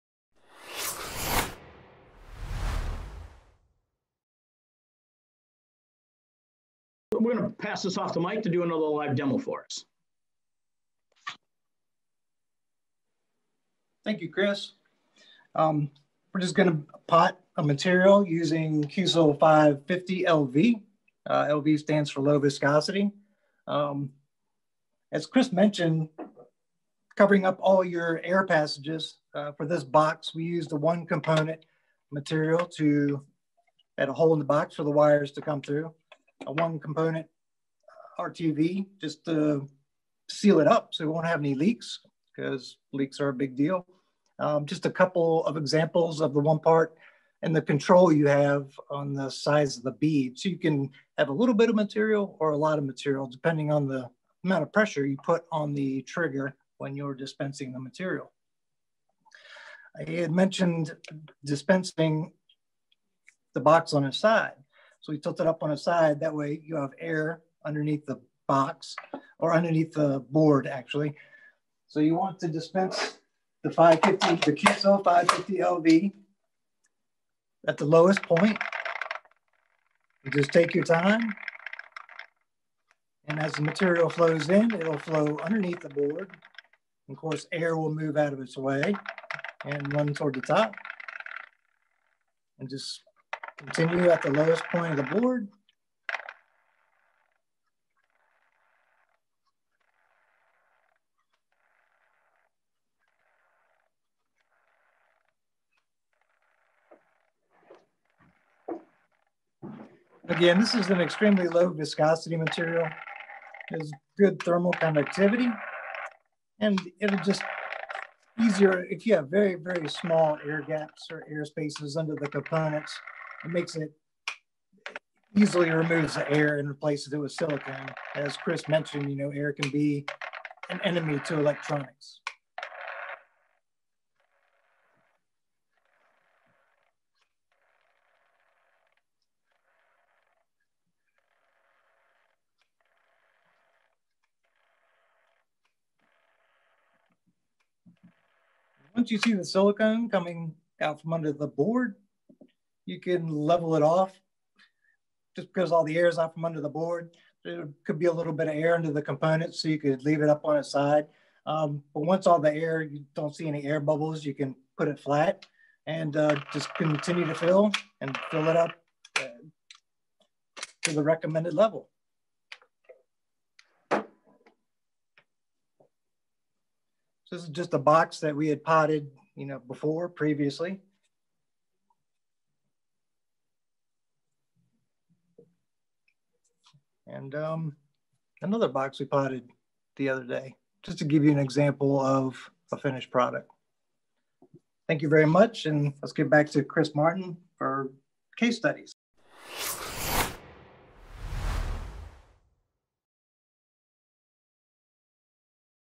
CHT Live Demo: Potting using QSil 550LV
Chemical Concepts and CHT USA hosted an informative webinar to discuss the application of silicone-based compounds in the manufacturing and installation of LED assemblies, and other electronic components.

Listen to application experts from CHT and Chemical Concepts explain how silicones can be used to protect LEDs from the elements, manage heat, and maintain optimal light transmission in the toughest environments, even underwater! This webinar also included a Q&A portion and a live interactive demonstration broadcasted directly from CHT's lab in Richmond, VA

Topics covered include:

-How to Protect LEDs from the Environment
-Optimizing Light Transmission
-Casting Your Own Lenses
-How to Select and Apply Silicone Compounds
-Best Practices and Frequently Asked Questions